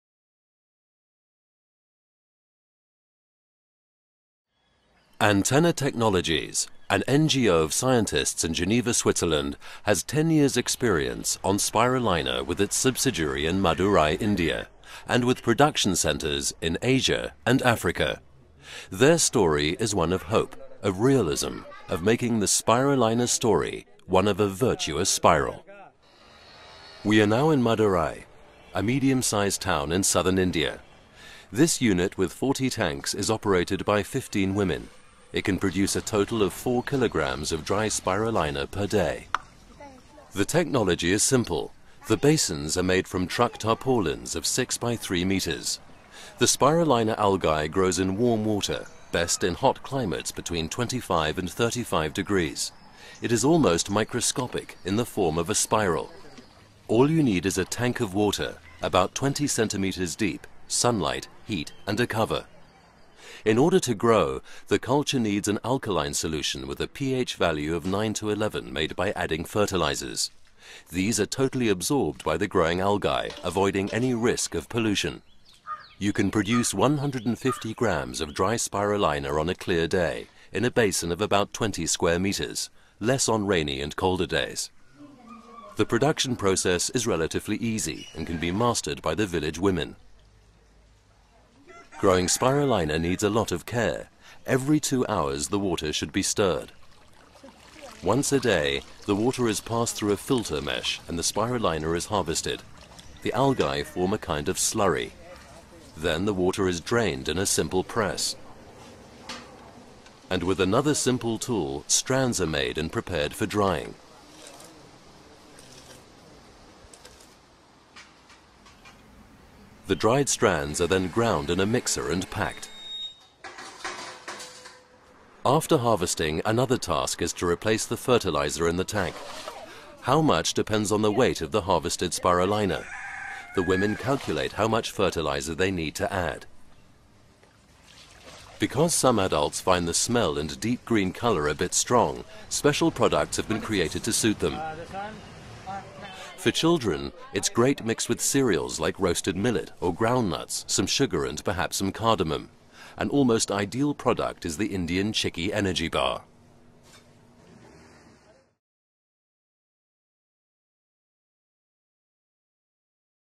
Visit a local culture of spirulina in Madurai
Antenna Foundation started conducting research on spirulina in 1992 and has been active in the local production and distribution of the highly nutritional micro-alga ever since. It has been shown by field trials that a daily dose of just a few grams of spirulina can lead to spectacular improvements in the nutritional state of malnourished children.

Here you can visit a culture of spirulina in Madurai, India. The farm has changed since the video was shot, back in 2007, but you can learn how spirulina is locally produced and prepared for consumption at an ecoparc run by our partner Antenna Trust.

Spirulina is no long-term solution to overcome it, but offers great potential in the fight against chronic malnutrition. As a result, Antenna has taken the decision to link awareness-raising and the prevention of infant malnutrition on the one hand, with local production and consumption of spirulina on the other, also creating a sustainable economic activity for members of the local community.

Currently, Antenna Foundation is testing a new technique for producing spirulina in closed tanks (instead of the open ponds that can be seen in the video) in Ghana, Brazil and India.

For more information about Antenna's activities in the area of nutrition, please visit our

For the work of Antenna France in this area (mainly in Africa)